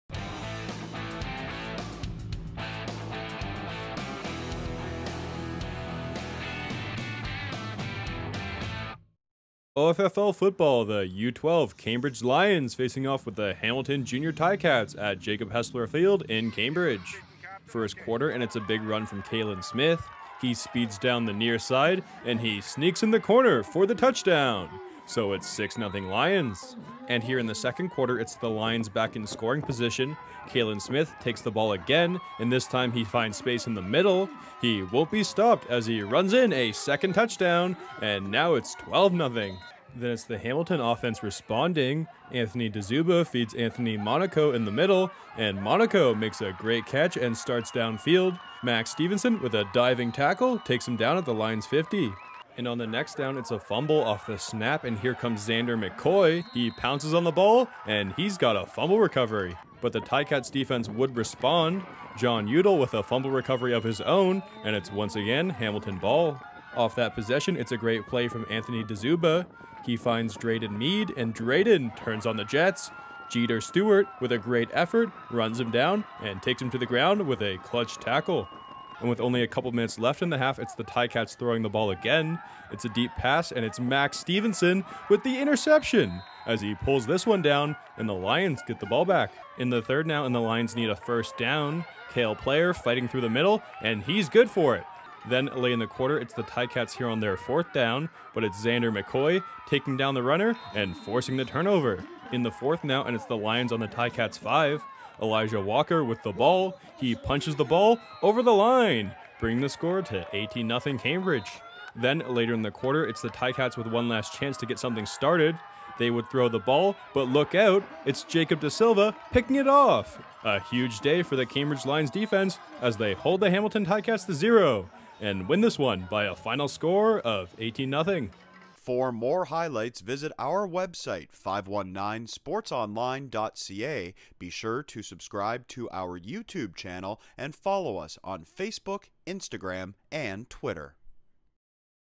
Under-12 OFFL Football - Hamilton Jr. Tiger Cats vs Cambridge Lions
Highlights from Saturday October 1st between the Ti-Cats and Lions at Jacob Hespeler Secondary School.

This video is presented by Cambridge Minor Football. Learn more about their tackle and flag programs by visiting